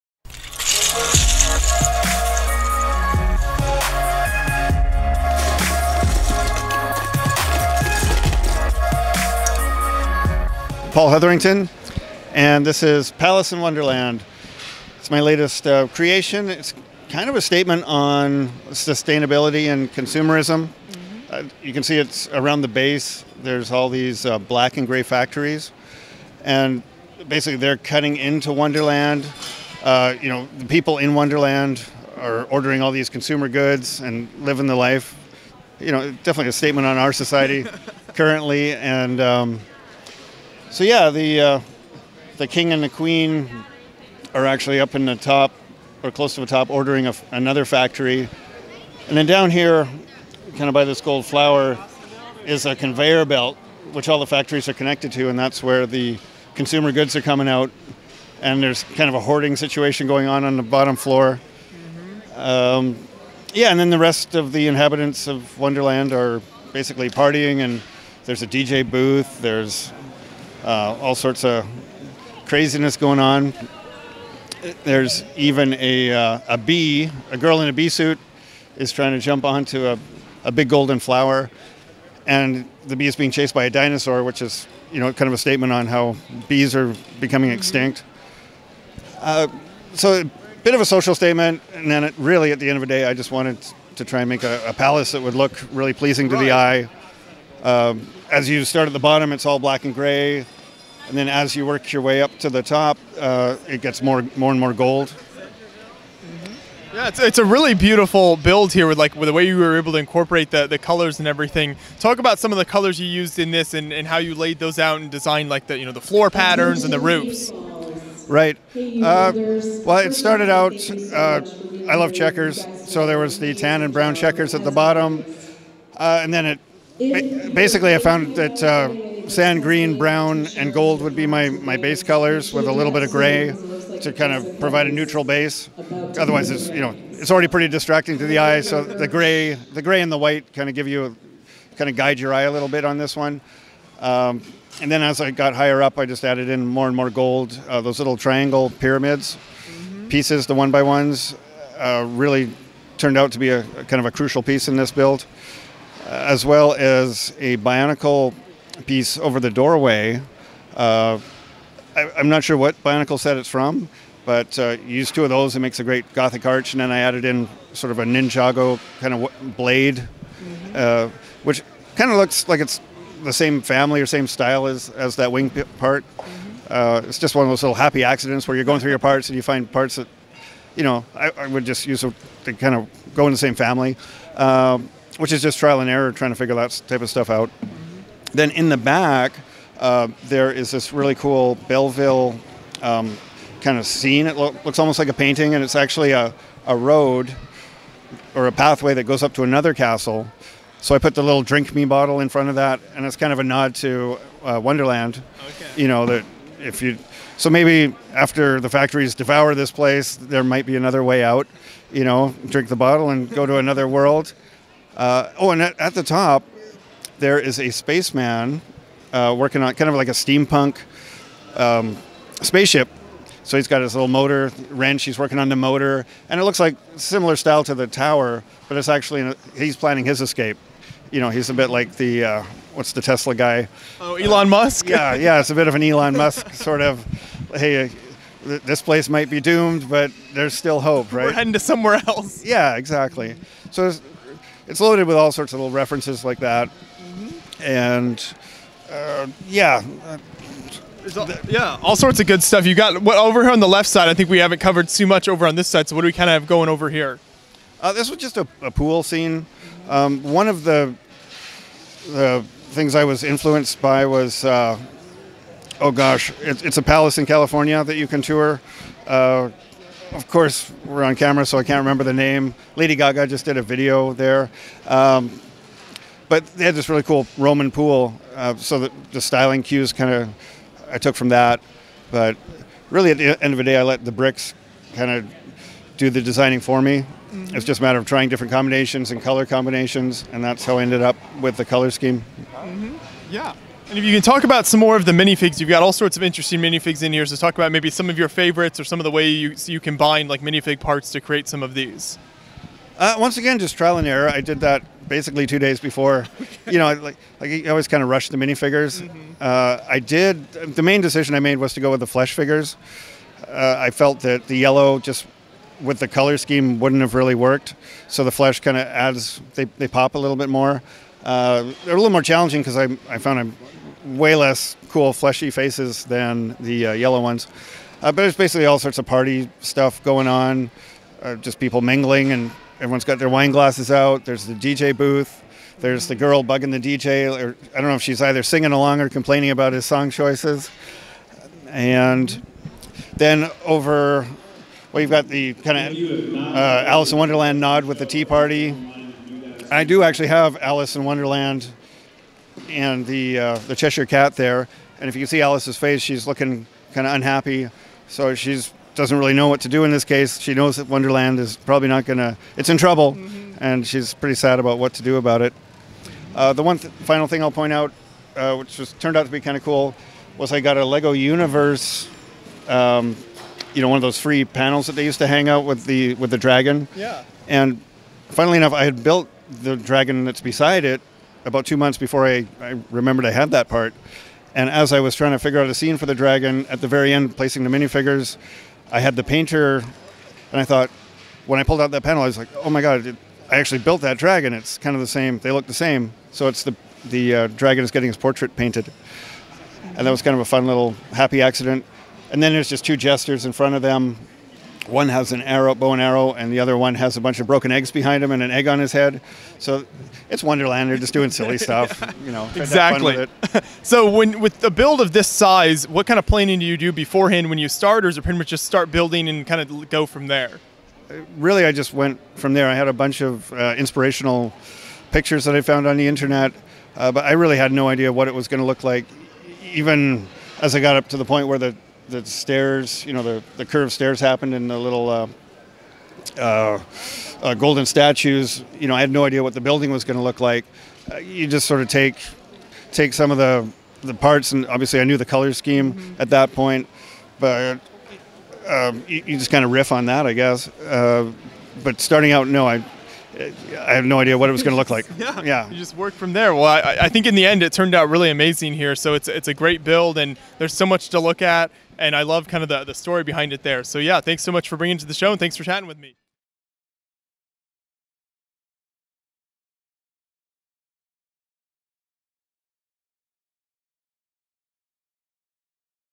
LEGO Palace in Wonderland | Bricks Cascade 2018
Beyond the Brick's Joshua Hanlon talks with Paul Hetherington about his LEGO Palace in Wonderland creation at Bricks Cascade 2018.